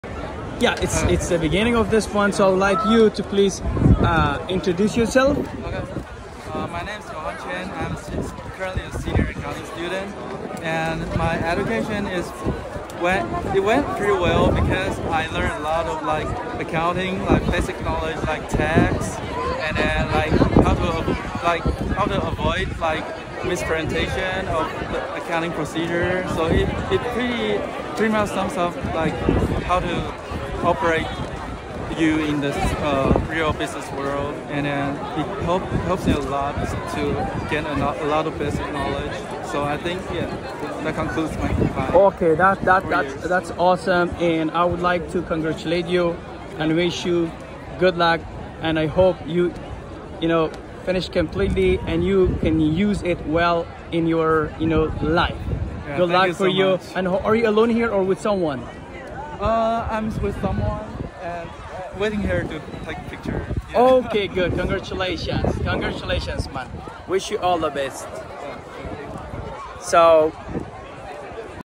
Short interview with the UB Graduate 👨‍🎓 Sturent in University at Buffalo NY USA
A short interview with the UB graduate student.
I congratulate and wish them the best of their achievements. 🎊👩‍🎓🙏🎈

جشن فراغت محصلین دانشگاه یوبی بود ‌و از چند محصل‌محاحبه داشتم‌ بنآ این دست آورد و‌فراغت را برای‌ همه شان تبریک‌ میگویم و آرزوی موفقیت و‌دست بیشتر در امور زندگی‌ایشان‌ دارم.